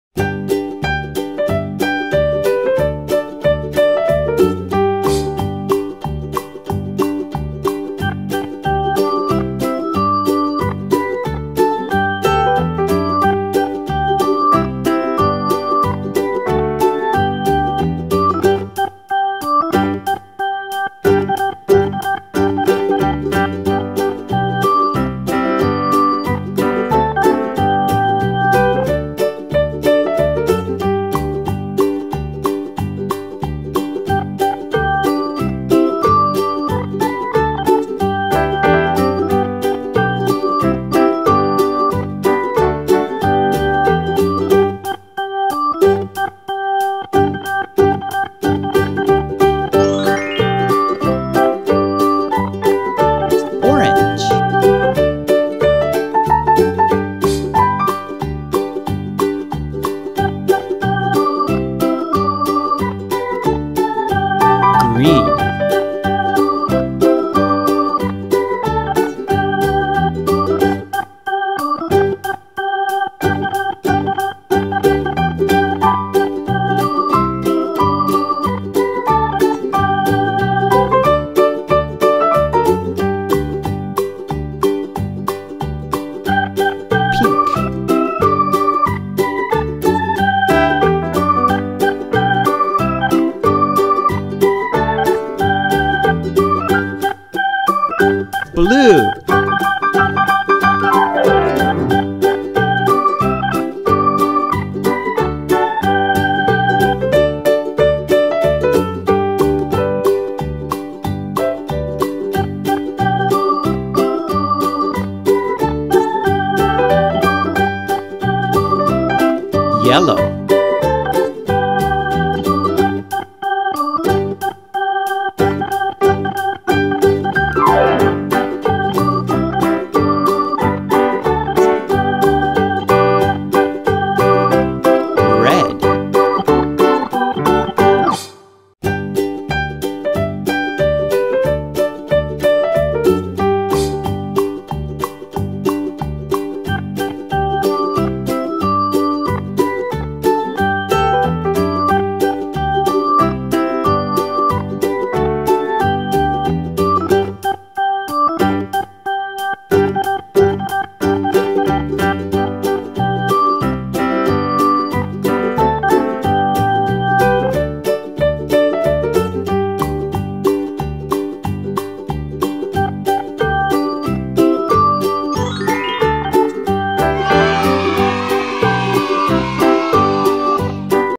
Cara Menggambar dan Mewarnai Truk Pengaduk untuk Anak dengan Guas
Menggambar dan mewarnai truk pengaduk untuk anak